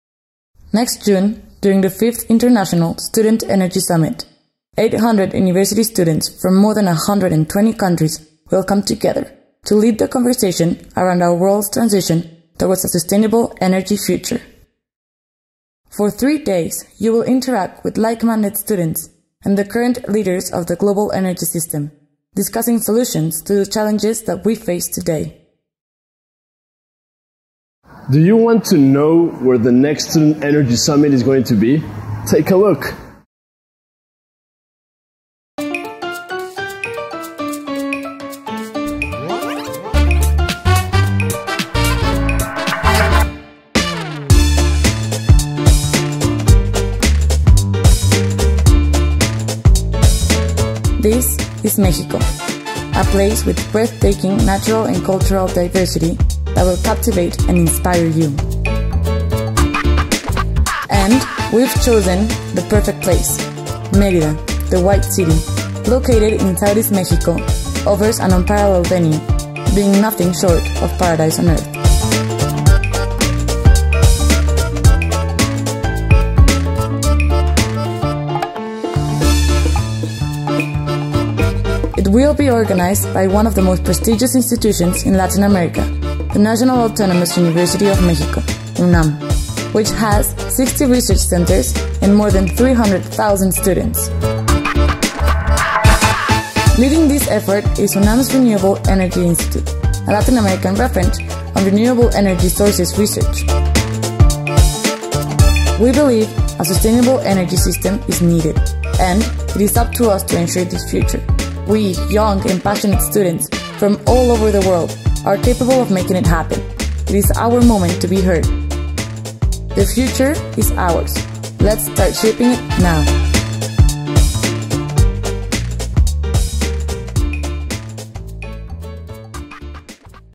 SES 2017 Launch Video
International Student Energy Summit 2017

Mérida, México

The International Student Energy Summit (SES) is a global forum that focuses on sustainable resource management and the role that students will play in defining the future of energy development.

Credits to La Palida Productora